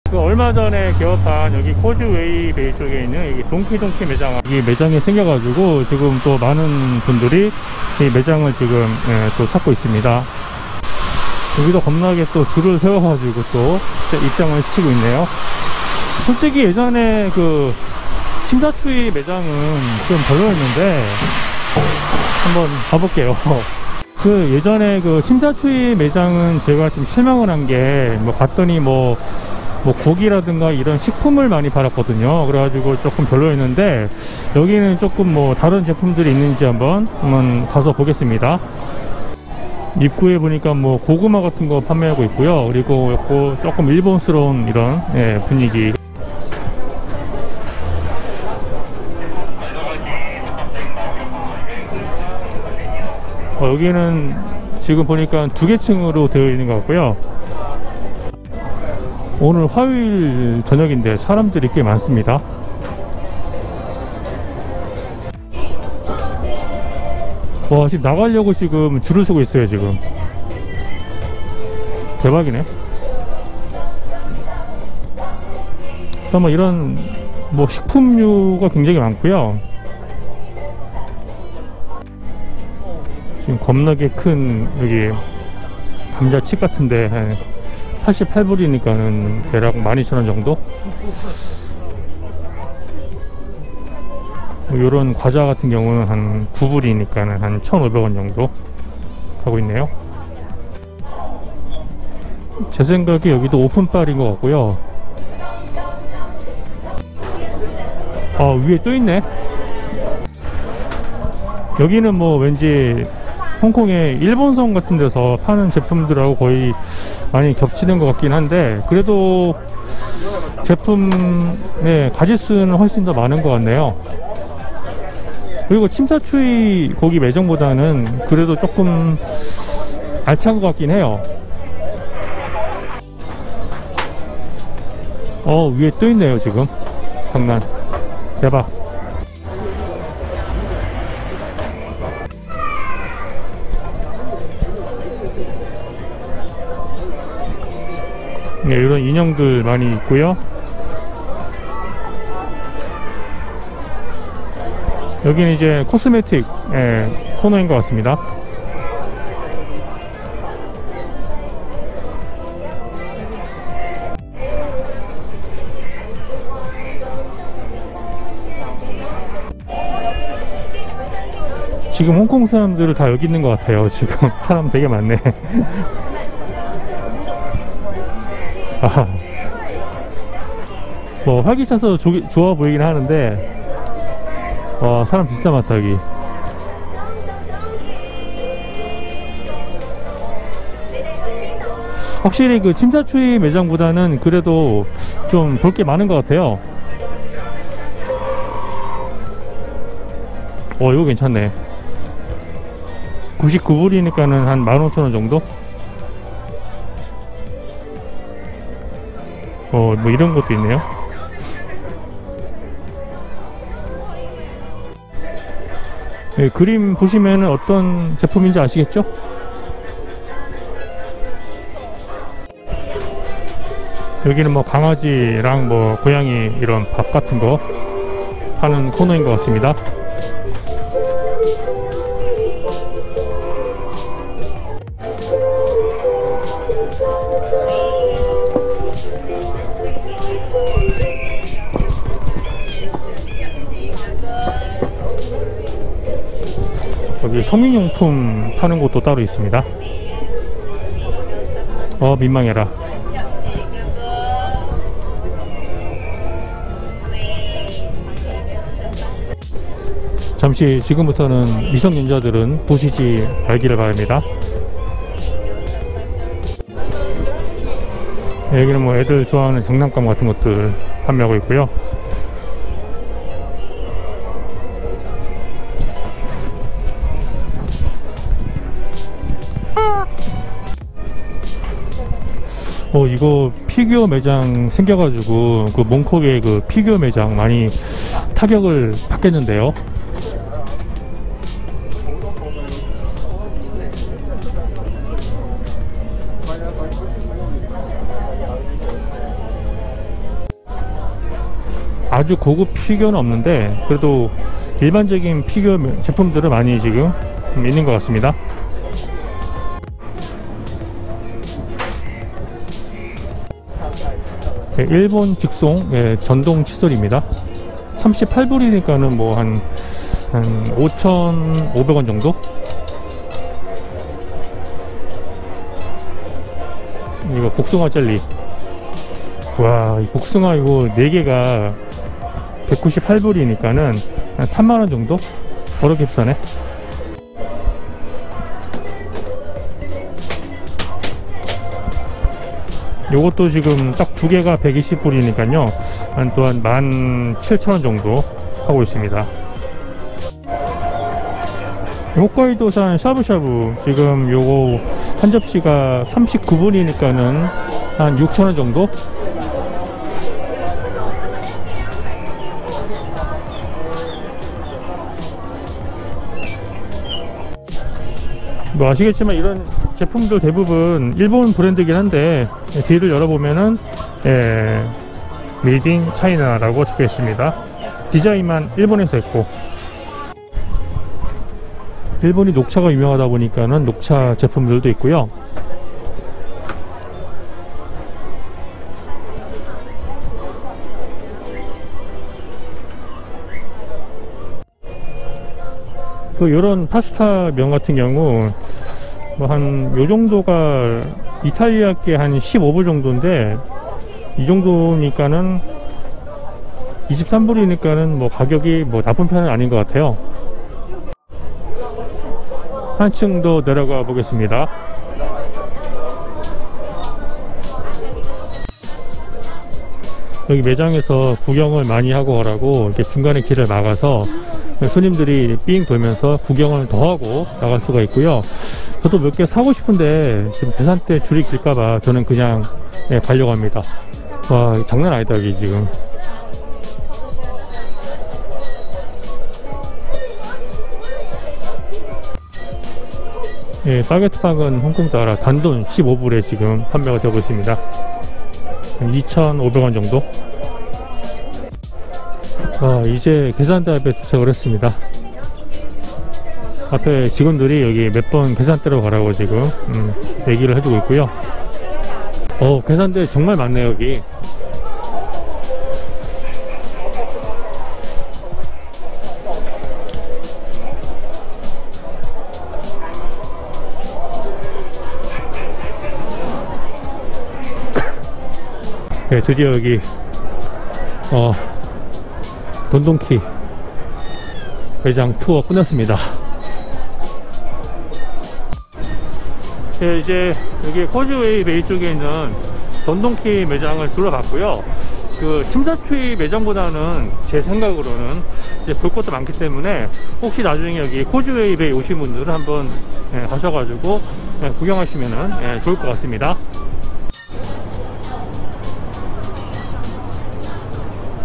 코즈웨이베이 돈 돈 돈키 매장 | 홍콩 쇼핑 랜선 투어
새로 오픈한 홍콩 코즈웨이 베이 돈 돈 돈키 매장에 다녀왔습니다.

안녕하세요!!!